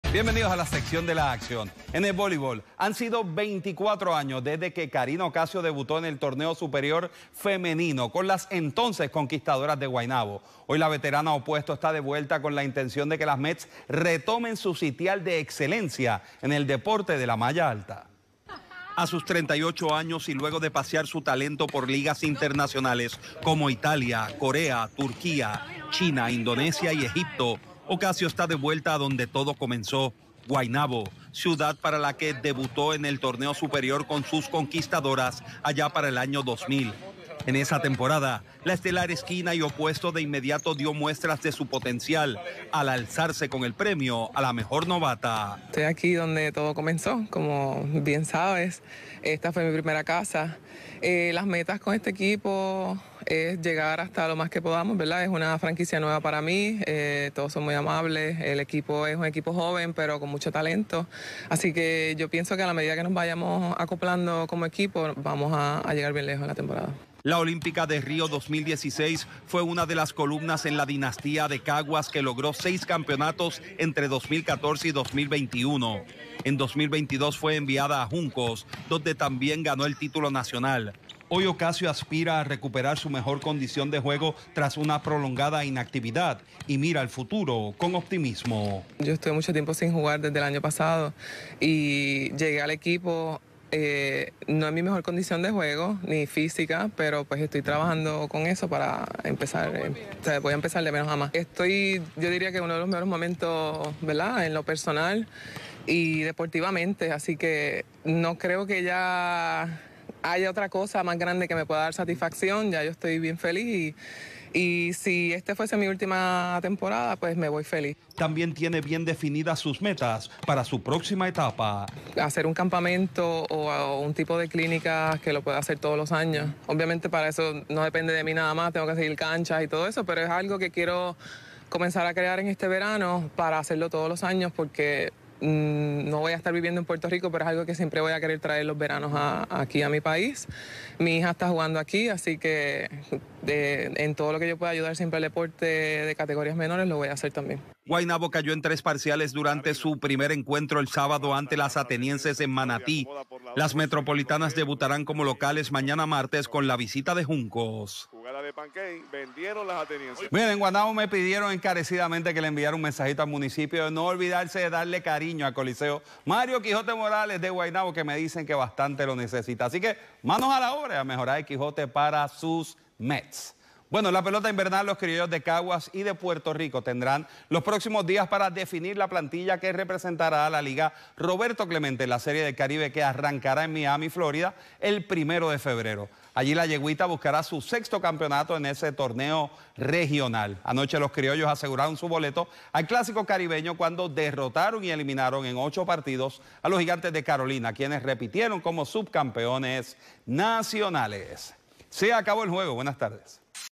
De regreso a la cancha la veterana Karina Ocasio
Con altas metas para las Mets, donde todo comenzó.